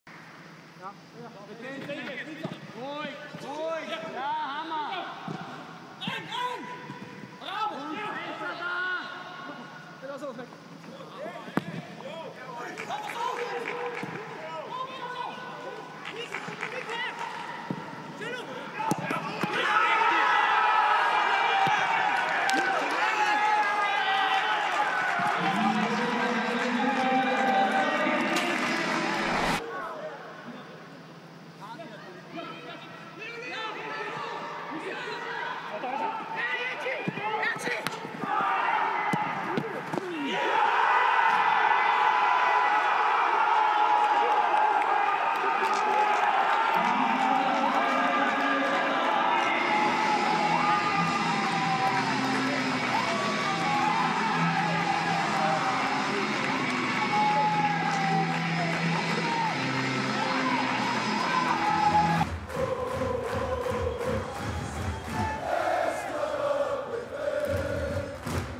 Grün-weiße Torparade: SK Rapid vs. Wolfsberger AC | Tipico Bundesliga 26. Runde
Auch das dritte Spiel hintereinander geht an uns: Der WAC wurde im Allianz Stadion mit 2:1 niedergerungen! Die Tore von Koya Kitagawa und Maximilian Ullmann im Schnelldurchlauf. Auf Rapid TV findet ihr viele weitere Videos rund um das gestrige Spiel gegen den WAC.